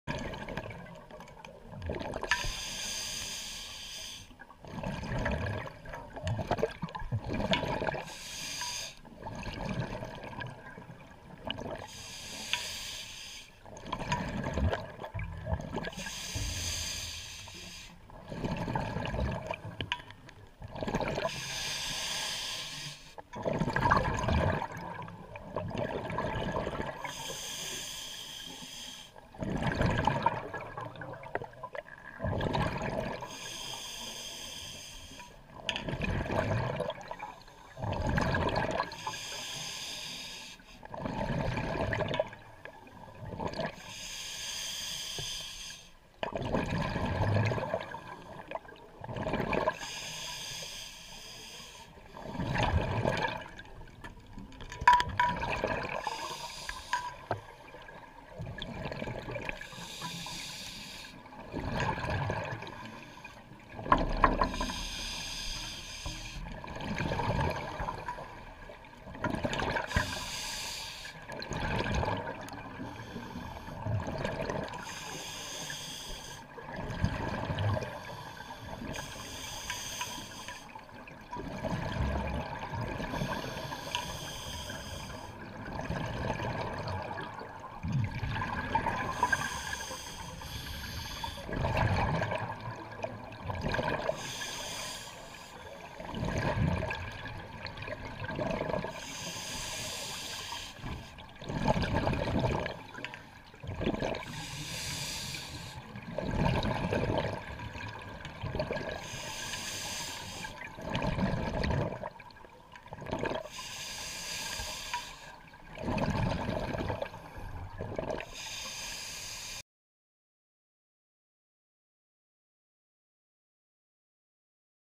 De Beldert 25-02-2015
Duik bij De Beldert,rond de kajuitjacht.